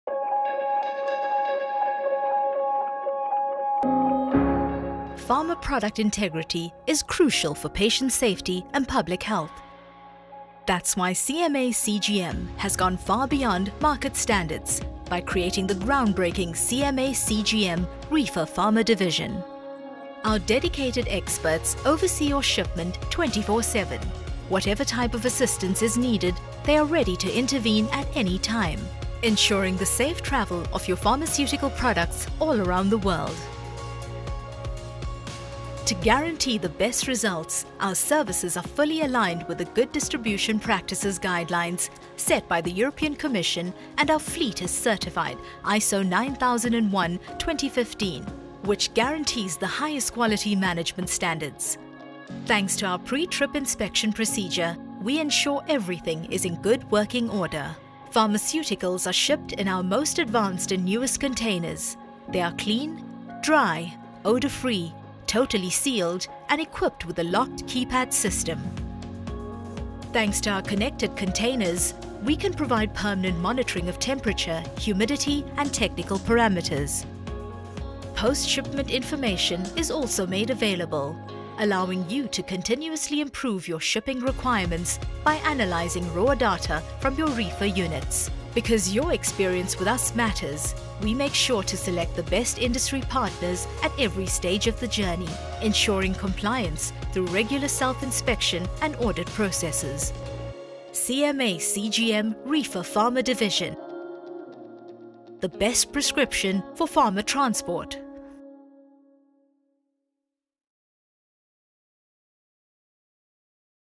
CMA CGM Pharma Reefer Division
The preservation of product integrity during transport is a fundamental priority, especially for pharmaceutical products when public health and patient safety are at stake.

Our dedicated experts are available 24/7 and the service they deliver meets the highest level of international standards. Our service is compliant with the Good Distribution Practice recommended by the European Union and our vessel fleet is ISO9001:2015 certified. We use the most recent reefer containers, with secured procedures, a transparent live monitoring of container temperature and technical parameters, not forgetting regular audits, self-inspections and selection of the best in industry business partners and subcontractors.

Thus, we can guarantee our customers their products will benefit from the best care all the way.